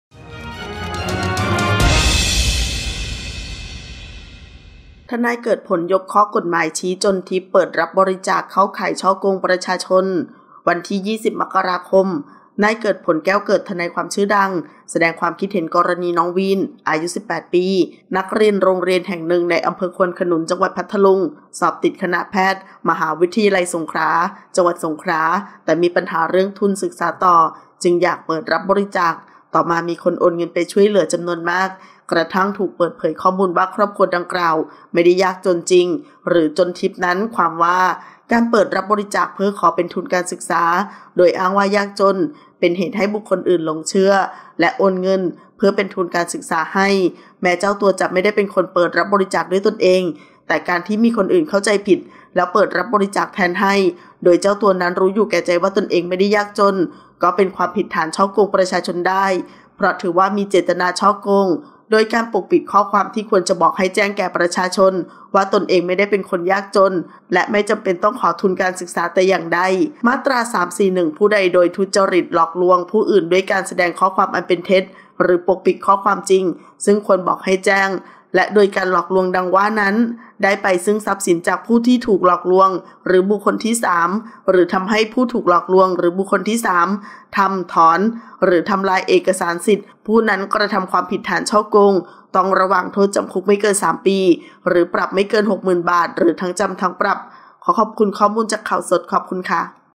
ทนายเกิดผล ยกข้อ กม. ชี้ จนทิพย์ เปิดรับบริจาค เข้าข่ายฉ้อโกงประชาชน
วันที่ 20 ม.ค.2566 นายเกิดผล แก้วเกิด ทนายความชื่อดัง แสดงความคิดเห็น กรณี น้องวีน อายุ 18 ปี นักเรียนโรงเรียนแห่งหนึ่งใน อ.ควนขนุน จ.พัทลุง สอบติดคณะแพทย์ศาสตร์ ม.สงขลานครินทร์ จ.สงขลา แต่มีปัญหาเรื่องทุนศึกษาต่อ จึงอยากเปิดรับบริจาค ต่อมาก็มีคนโอนเงินไปช่วยเหลือจำนวนมาก กระทั่งถูกเปิดเผยข้อมูลว่าครอบครัวดังกล่าวไม่ได้ยากจนจริง หรือ จนทิพย์ นั้น ความว่า

การเปิดรับบริจาคเพื่อขอเป็นทุนการศึกษา โดยอ้างว่ายากจน เป็นเหตุให้บุคคลอื่นหลงเชื่อ และโอนเงิน เพื่อเป็นทุนการศึกษาให้ แม้เจ้าตัวจะไม่ได้เป็นคนเปิดรับบริจาคด้วยตนเอง แต่การที่มีคนอื่นเข้าใจผิด แล้วเปิดรับบริจาคแทนให้

โดยเจ้าตัวนั้นรู้อยู่แก่ใจว่าตนเองไม่ได้ยากจน ก็เป็นความผิดฐานฉ้อโกงประชาชนได้ เพราะถือว่า มีเจตนาฉ้อโกง โดยการปกปิดข้อความจริงที่ควรบอกให้แจ้งแก่ประชาชน ว่าตนเองไม่ได้เป็นคนยากจนและไม่จำเป็นต้องขอทุนการศึกษา แต่อย่างใด

มาตรา 341 ผู้ใดโดยทุจริต หลอกลวงผู้อื่นด้วยการแสดงข้อความอันเป็นเท็จ หรือปกปิดข้อความจริงซึ่งควรบอกให้แจ้ง และโดยการหลอกลวงดังว่านั้น ได้ไปซึ่งทรัพย์สินจากผู้ถูกหลอกลวงหรือบุคคลที่สาม หรือทำให้ผู้ถูกหลอกลวงหรือบุคคลที่สาม ทำ ถอน หรือทำลายเอกสารสิทธิ ผู้นั้นกระทำความผิดฐานฉ้อโกง ต้องระวางโทษจำคุกไม่เกินสามปี หรือปรับไม่เกินหกหมื่นบาท หรือทั้งจำทั้งปรับ

00:00 - Intro
00:10 - News
01:00 - Minute